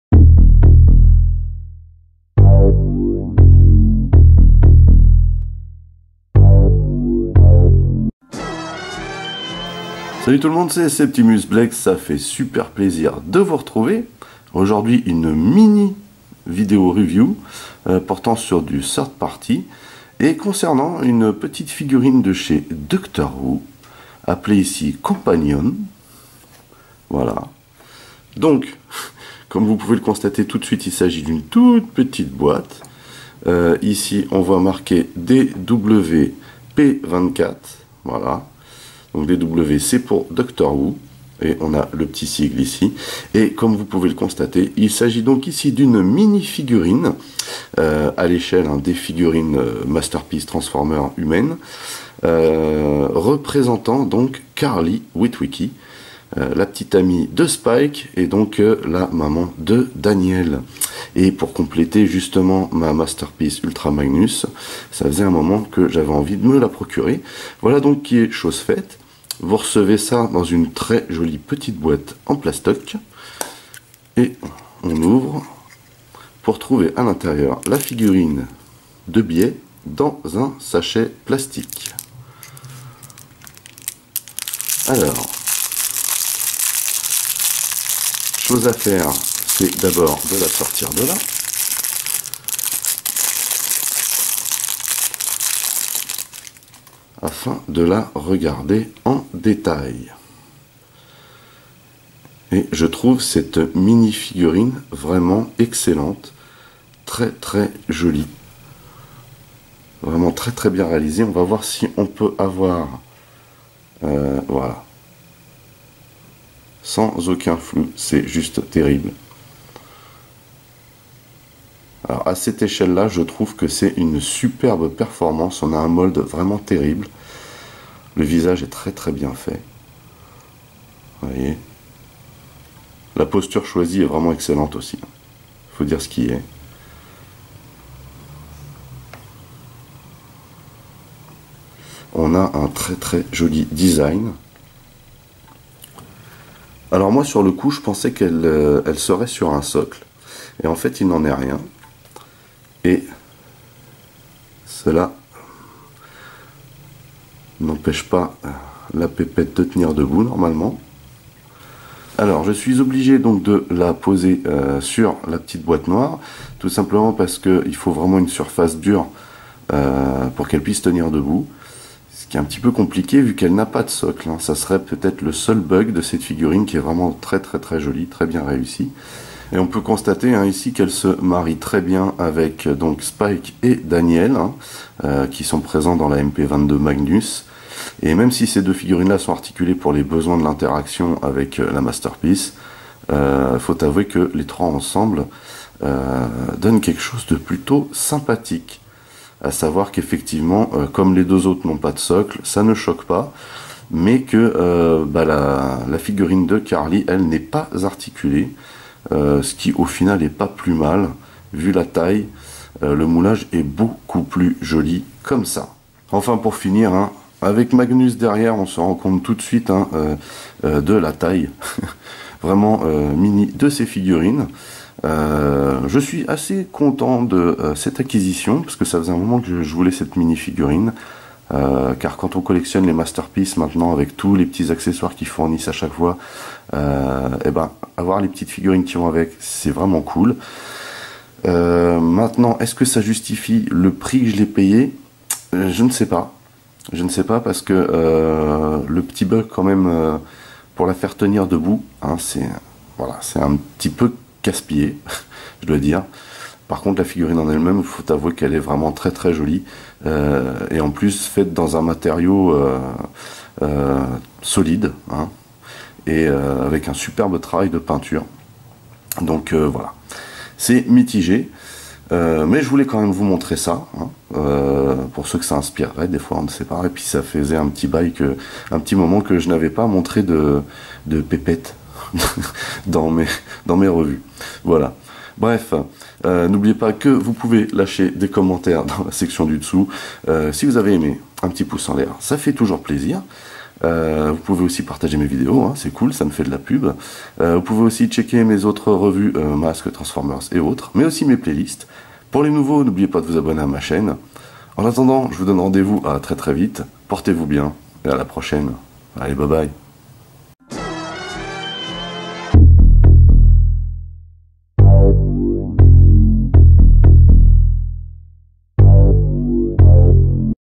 Zblex French Reviews : Doctor Wu DW-P24 Companion
Une Mini Vidéo "Review" en Français de la mini figurine de Doctor Wu à l'effigie de Carly. Bon visionnage !